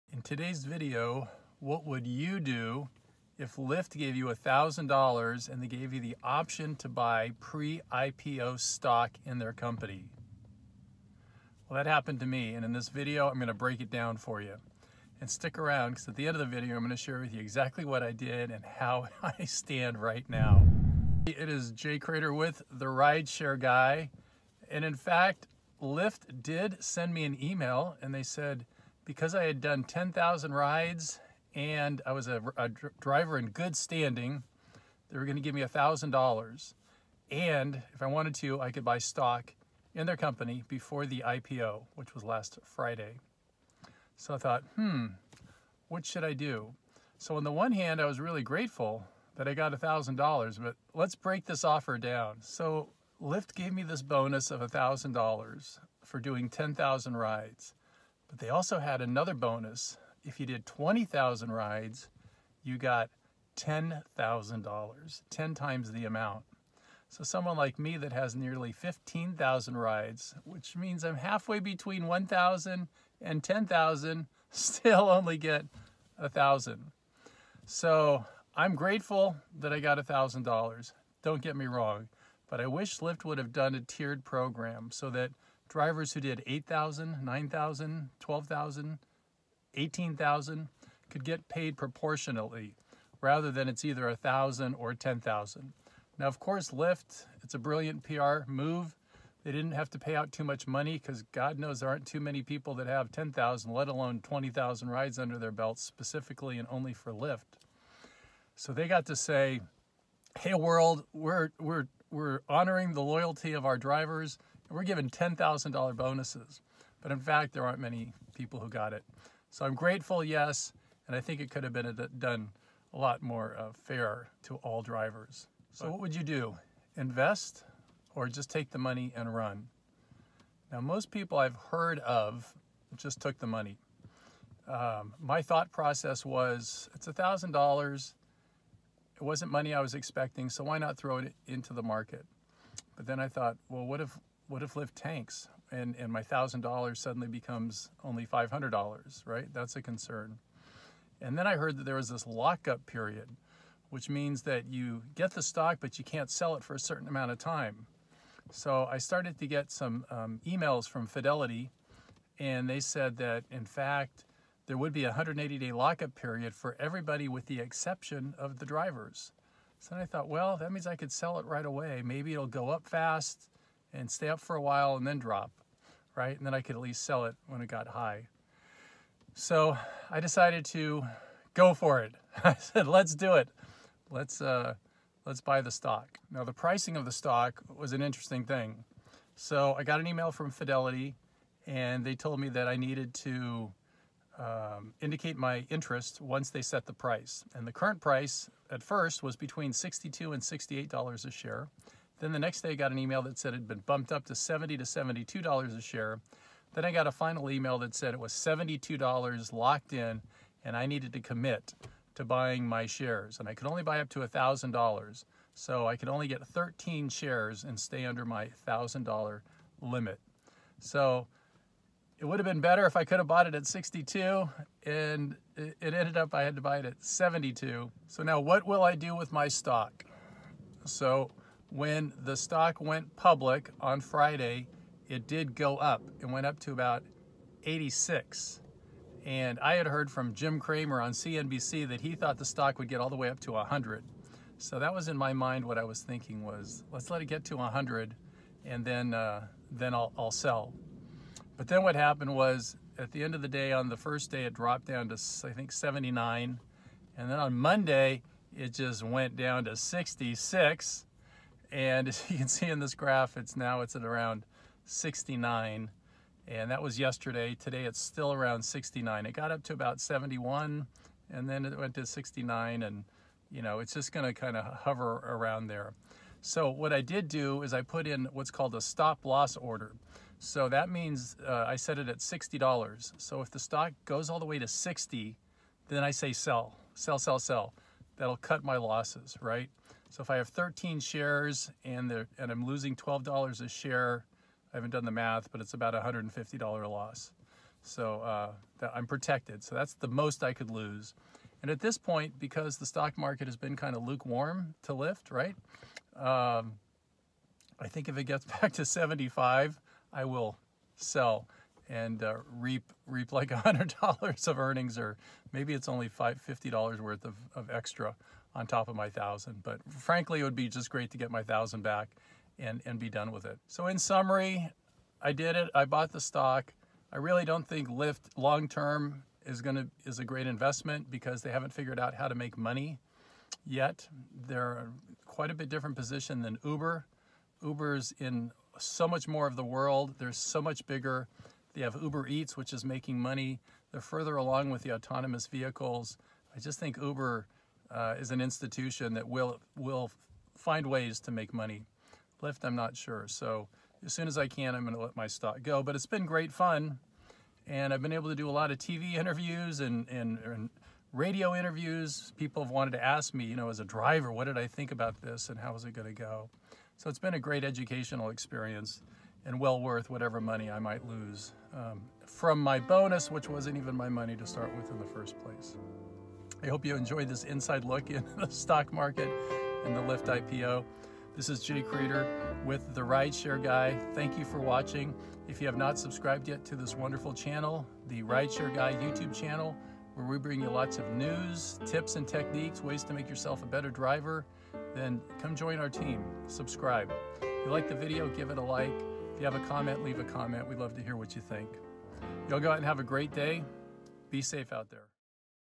I Bought $1,000 in Lyft IPO Stock and Here's What Happened!
Lyft recently handed out $1,000 cash bonuses to loyal Lyft drivers who had done over 10,000 rides and gave them the ability to buy into the IPO. Jay was one of those lucky drivers and bought $1,000 in Lyft stock on IPO day - find out what happened to his money!